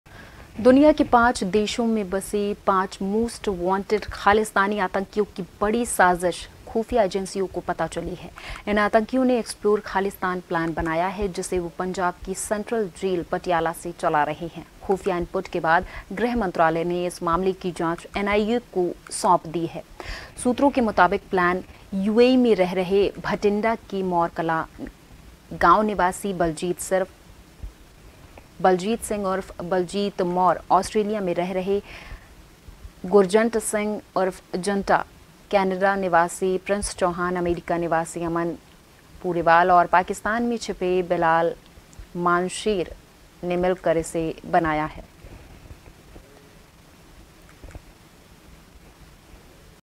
5 देशों में बैठे खालिस्तनी आतंकियों का एक्स्प्लोर प्लान, ख़ुफ़िया इनपुट के बाद NIA जाँच शुरू | JANTV
Watch JAN TV on :
Tata Play DTH : 1185
Airtel DTH: 355
JIO Fiber: 1384